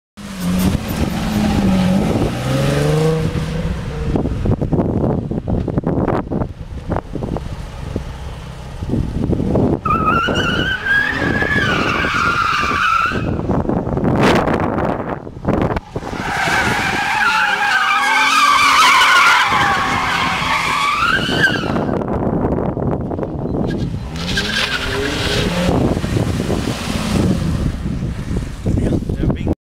350Z Burning some rubber
One of my friends having fun with the 350Z!!